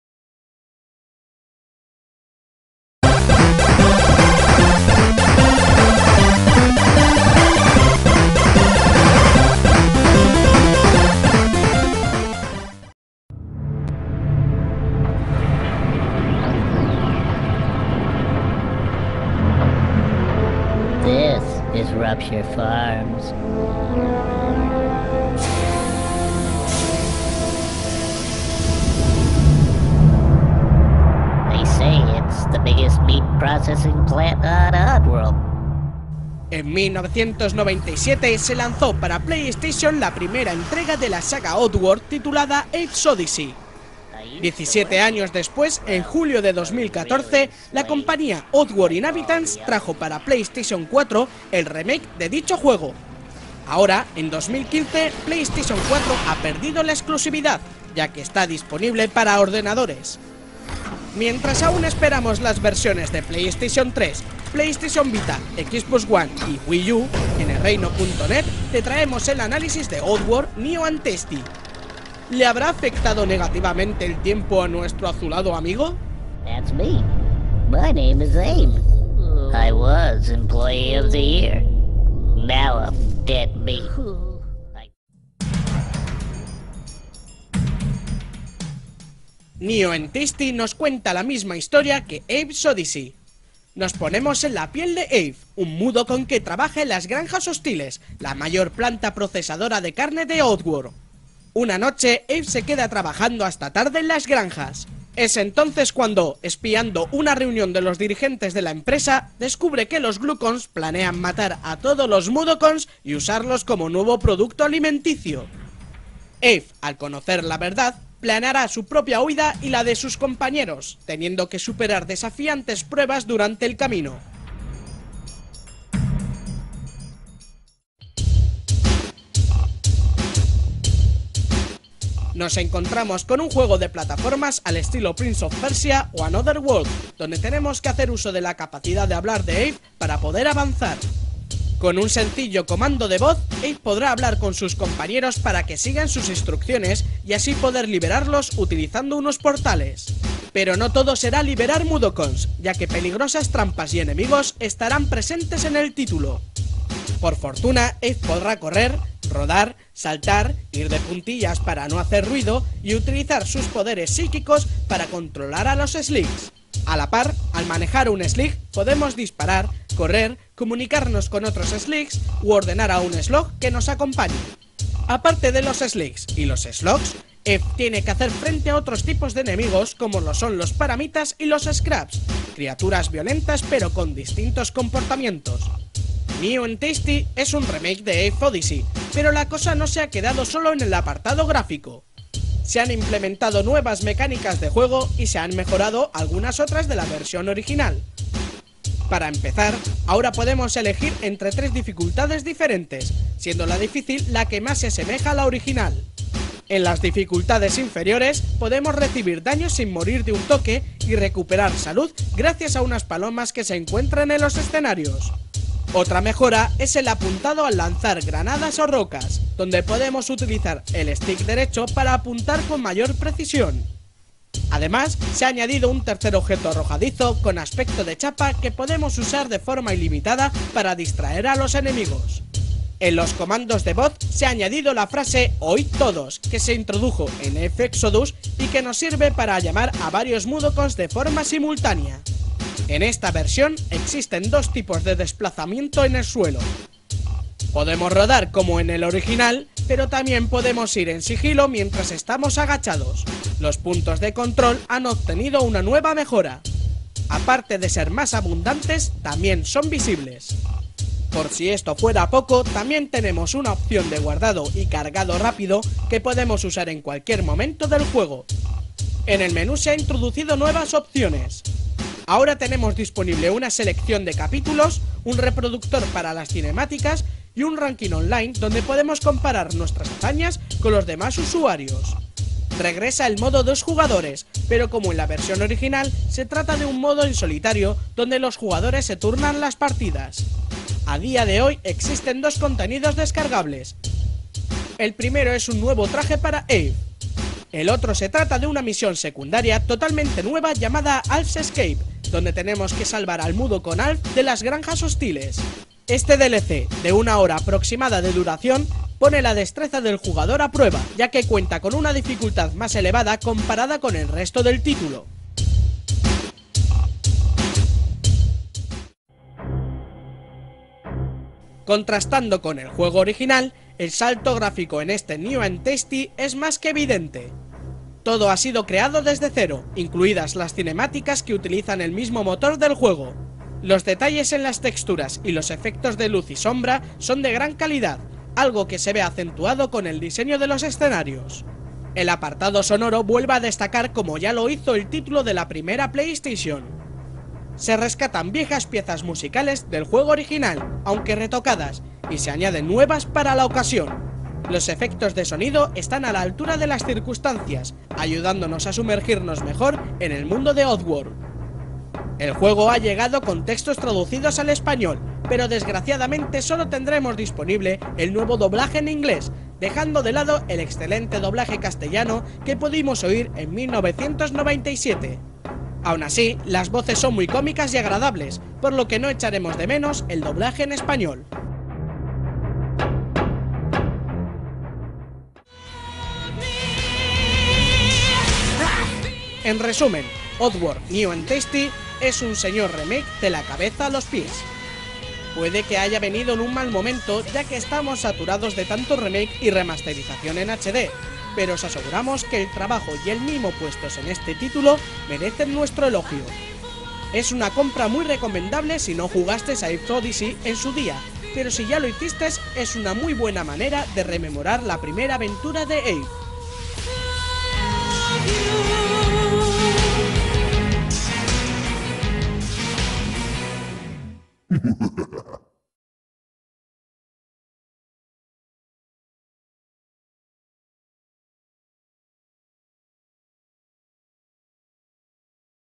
Análisis Oddworld: New 'n' Tasty! - MULTI
Vídeo Análisis Oddworld: New 'n' Tasty! para PlayStation 3, PC, PS Vita, PlayStation 4 y Xbox One.